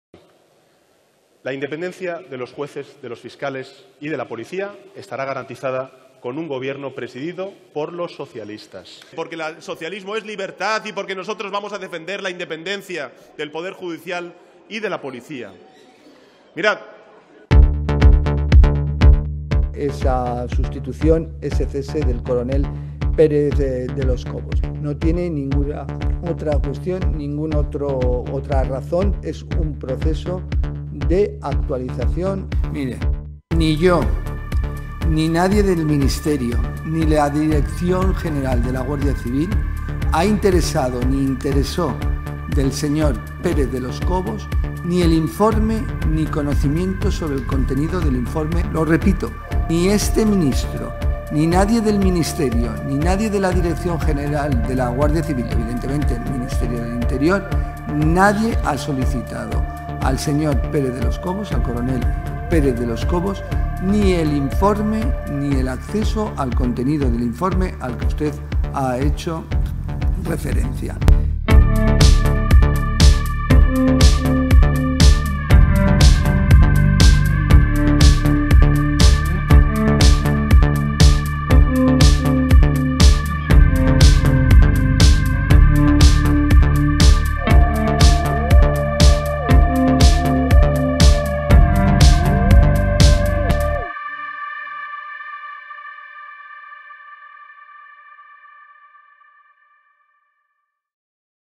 Pasen y vean las razones por las que Marlaska debe dimitir
El PSOE de Pedro Sánchez no tiene palabra. Dice una cosa y hace otra...